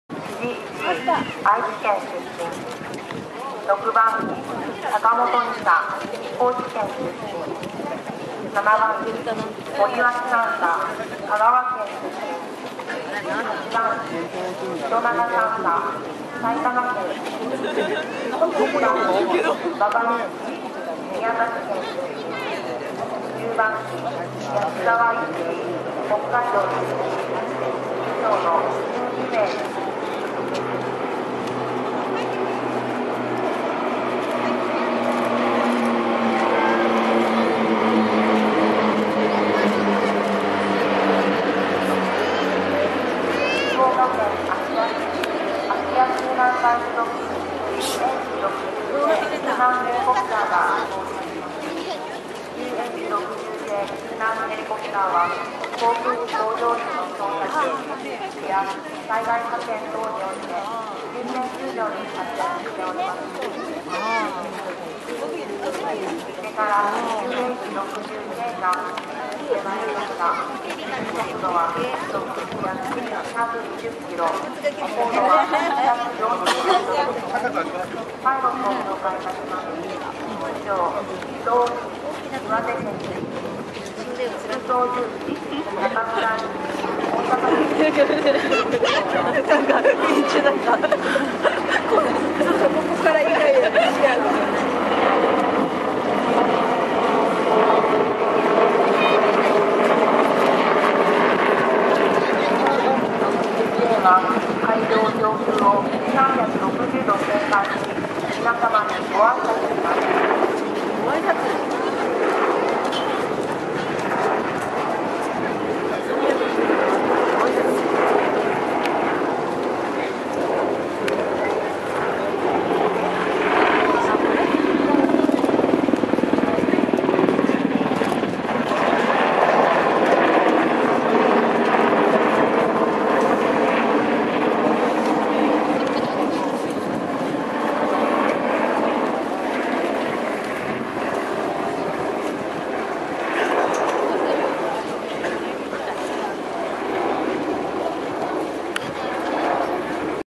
[20100606]空自x防府北x航過飛行-T-7&UH-60J-
山口県防府市にある航空自衛隊防府北基地の防府航空祭に行って来ました!
T-7編隊スゴい!UH-60Jは芦屋救難隊!!
#Japan Air Self Defense Force##